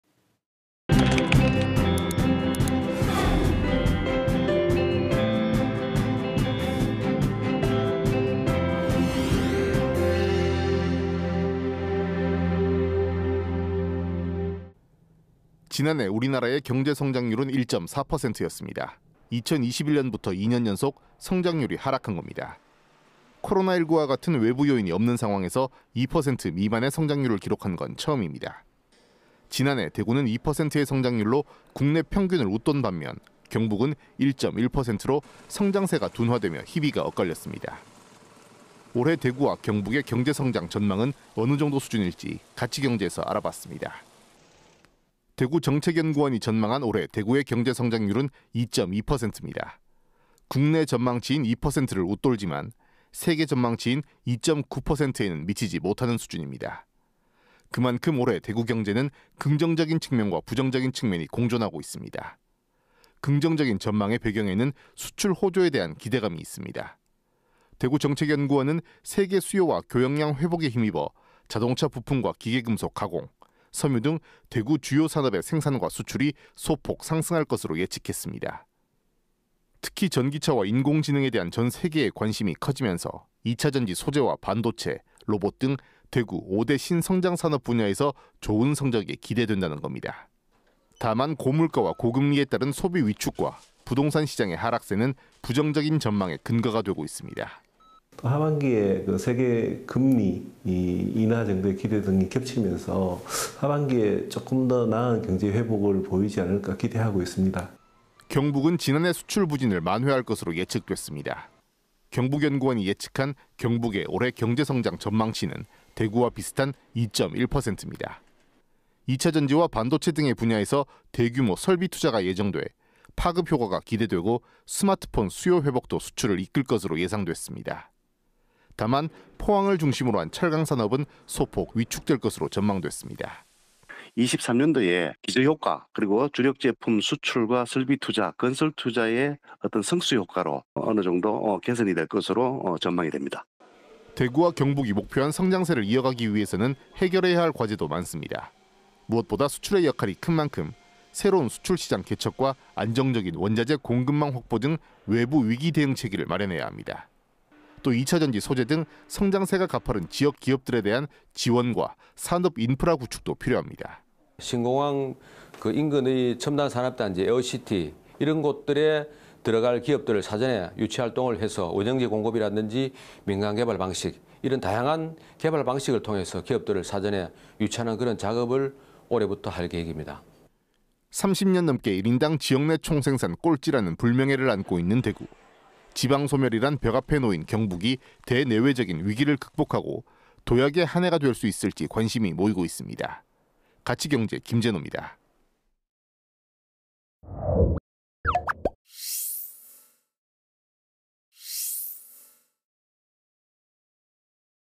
[같이경제] 대구·경북 올해 경제 전망과 과제는? / KBS 2024.01.31.
지난해 우리나라의 경제성장률은 1.4%였습니다.

 2021년부터 2년 연속 성장률이 하락한 겁니다.

 코로나19와 같은  외부 요인이 없는 상황에서  2% 미만의 성장률을 기록한 건 처음입니다.

 지난해 대구는 2%의 성장률로, 국내 평균을 웃돈 반면, 경북은 1.1%로 성장세가 둔화되며 희비가 엇갈렸습니다.

 올해 대구와 경북의  경제성장 전망은 어느 정도 수준일 지 같이 경제에서 알아봤습니다.

 대구정책연구원이 전망한 올해 대구의 경제성장률은  2.2%입니다.

 국내 전망치인 2%를 웃돌지만, 세계 전망치인 2.9%에는 미치지 못하는 수준입니다.

 그만큼 올해 대구 경제는 긍정적인 측면과 부정적인 측면이 공존하고 있습니다.

 긍정적인 전망의 배경에는 수출 호조에 대한 기대감이 있습니다.

 대구정책연구원은 세계 수요와 교역량 회복에 힘입어 자동차 부품과 기계금속 가공, 섬유 등 대구 주요 산업의  생산과 수출이 소폭 상승할 것으로 예측했습니다.

 특히 전기차와 인공지능에 대한 전 세계의 관심이 커지면서 2차 전지 소재와 반도체, 로봇 등 대구 5대 신성장 산업 분야에서 좋은 성적이 기대된다는 겁니다.

 다만, 고물가와 고금리에 따른 소비 위축과 부동산 시장의 하락세는  부정적인 전망의 근거가 되고 있습니다.

 [최재원/대구정책연구원 경제산업연구실 : "하반기에 세계금리 인하 정도에 대한 기대 등이 겹치면서 하반기에 조금 더 나은 경제 회복을 보이지 않을까 기대하고 있습니다."]

 경북은 지난해 수출 부진을 만회할 것으로 예측됐습니다.

 경북연구원이 예측한  경북의 올해 경제성장 전망치는  대구와 비슷한 2.1%입니다.

 2차 전지와 반도체 등의 분야에서 대규모 설비투자가 예정돼 파급 효과가 기대되고, 스마트폰 수요 회복도  수출을 이끌 것으로 예상됐습니다.

 다만 포항을 중심으로 한 철강산업은 소폭 위축될 것으로  전망됐습니다.

 [임규채/경북연구원 경제산업연구실장 : "23년도의 기저효과 그리고 주력 제품 수출과 설비투자, 건설투자의 승수효과로 어느 정도 개선이 될 것으로 전망이 됩니다."]

 대구와 경북이 목표한  성장세를 이어가기 위해서는 해결해야 할 과제도 많습니다.

 무엇보다 수출의 역할이 큰 만큼 새로운 수출시장 개척과  안정적인 원자재 공급망 확보 등 외부 위기 대응 체계를 마련해야 합니다.

 또 2차 전지 소재 등 성장세가 가파른 지역 기업들에 대한 지원과 산업 인프라 구축도  필요합니다.

 [김광묵/대구시 원스톱기업투자센터장 : "신공항 인근의 첨단산업단지, 에어시티 이런 곳들에 들어갈 기업들을 사전에 유치활동을 해서 원형지 공급이라든지 민간개발 방식 이런 다양한 개발방식을 통해서 기업들을 사전에 유치하는 그런 작업을 올해부터 할 계획입니다."]

 30년 넘게 1인당 지역내 총생산 꼴찌라는 불명예를 안고 있는 대구.

 지방소멸이란 벽 앞에 놓인 경북이 대내외적인 위기를 극복하고  도약의 한 해가 될 수 있을 지 관심이 모이고 있습니다.

 같이 경제 김재노입니다.

 촬영기자:김동욱/그래픽:김지현